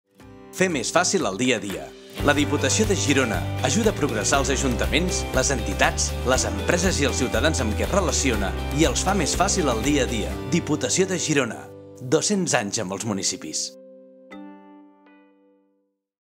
200 anys amb els municipis Falca ràdio Fer més fàcil (Veu Albert Niell)
Fer més fàcil el dia a dia!Nova promo que he enregistrat per la ràdio 200 anys amb els municipis de la diputació de Girona.